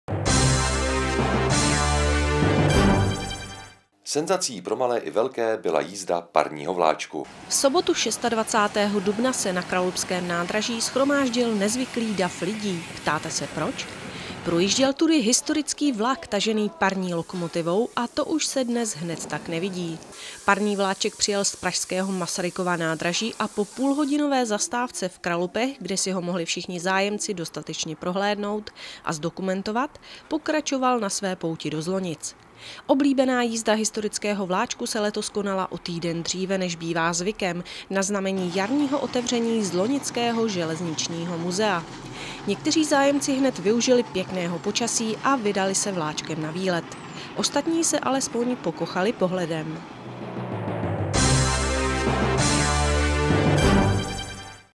Kralupy TV: Jízda historickým vláčkem (7. 5. 2008)
Jízda historickým vláčkem také v Kralupech.